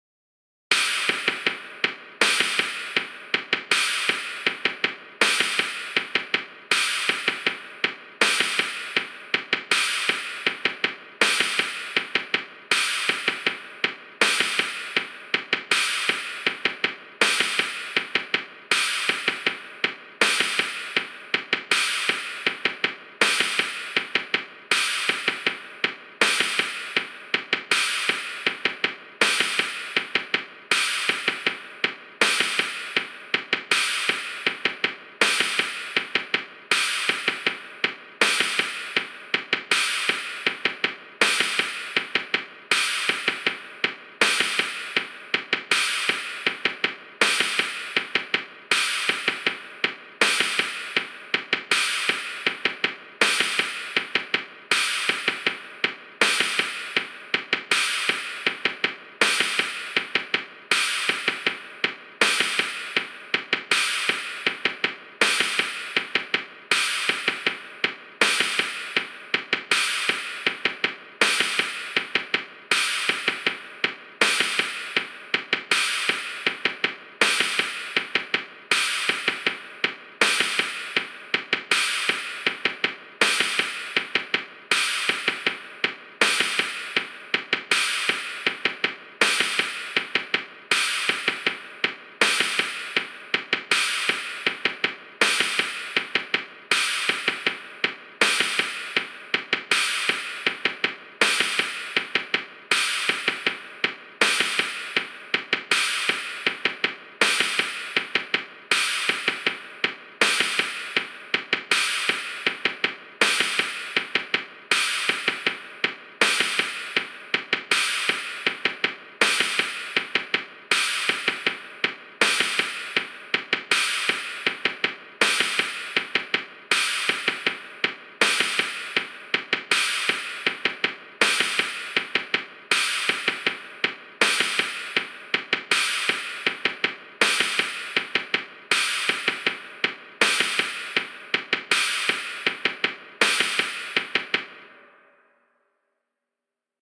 Kick coordination exercise 01, 40bpm
Here is a warmup to confuse the kick foot (or feet)!  Build the muscle memory here and then try the 80 bpm!